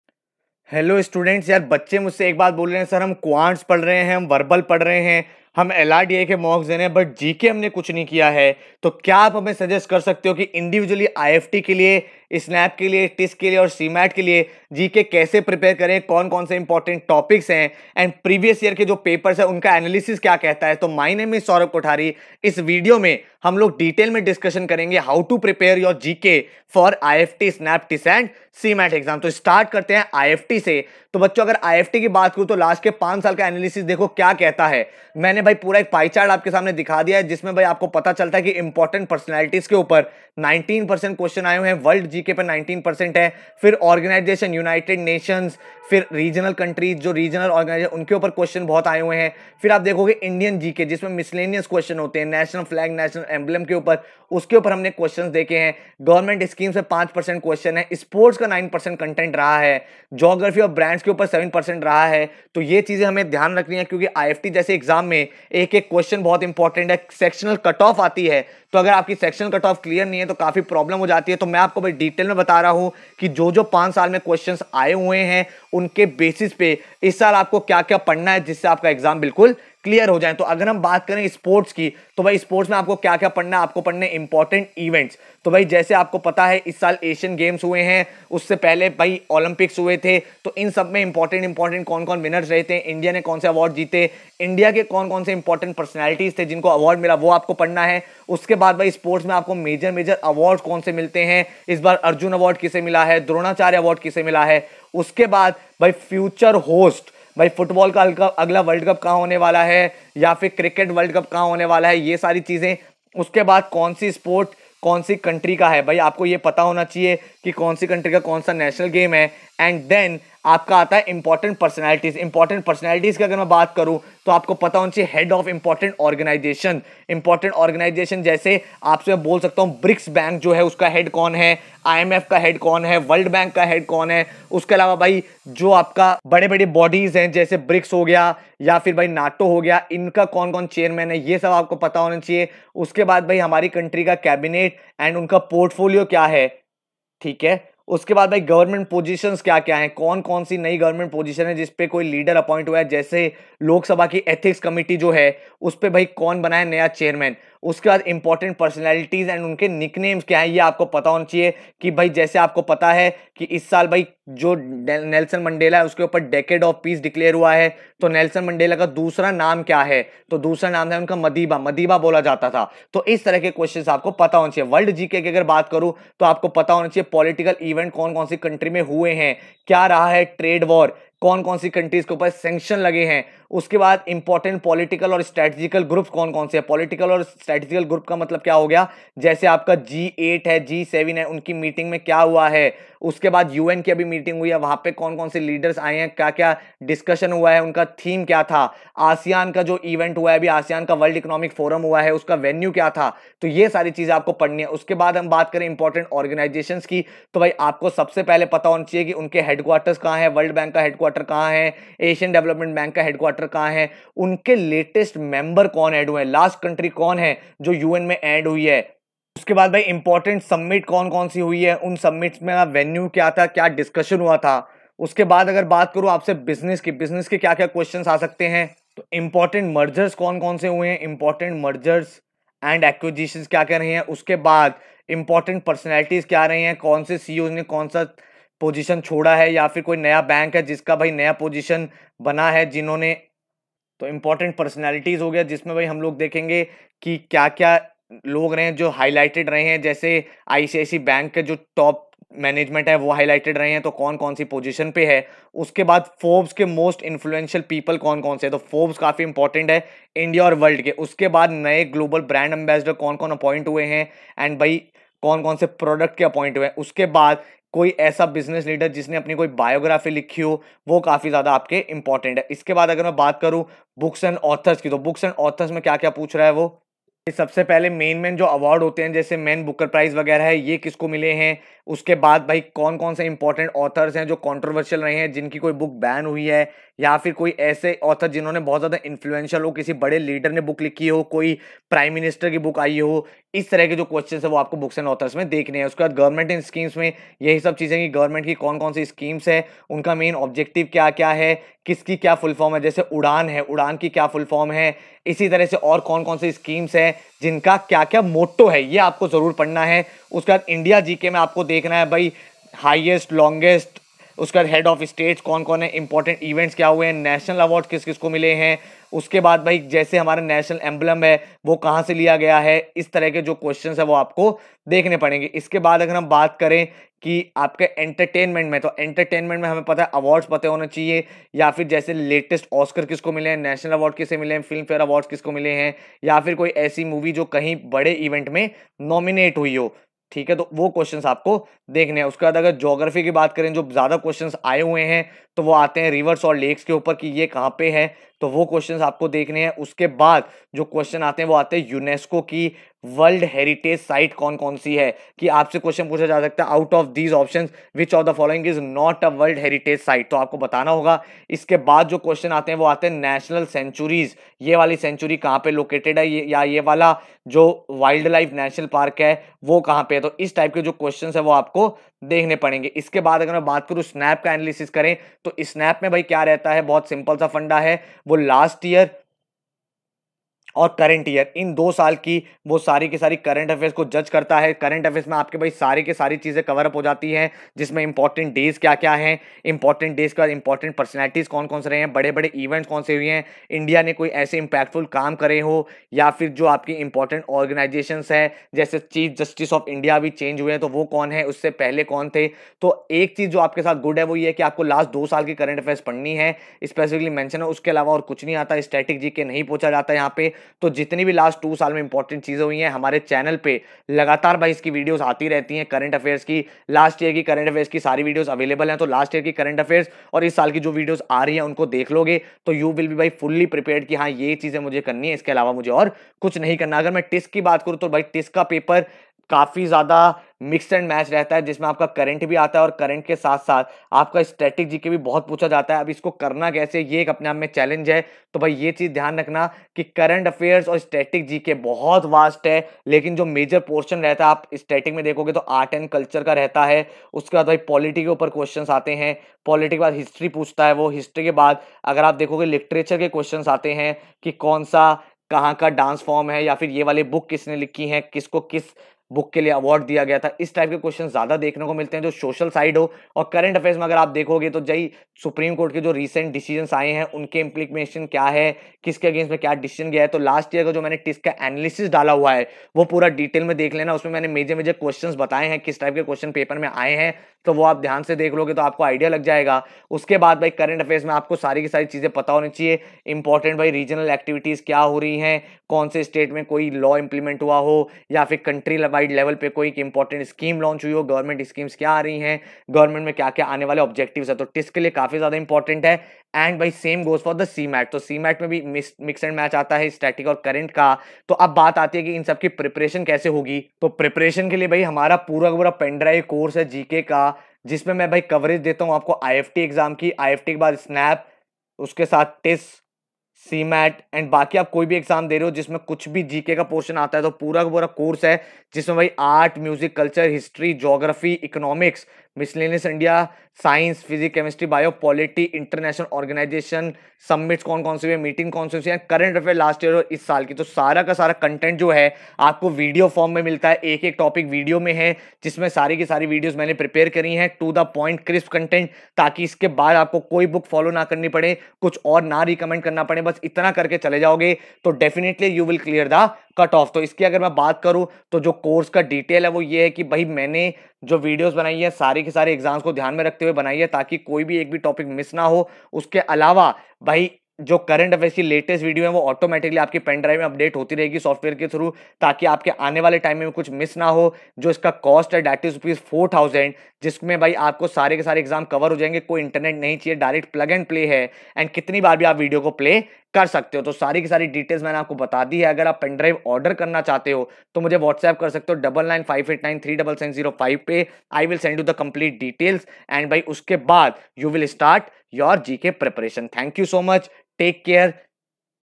How to prepare GK for IIFT, SNAP, TISS & CMAT exam? All important topics & questions
Find out the right strategy to prepare for general knowledge section for IIFT, SNAP, CMAT & TISS exam.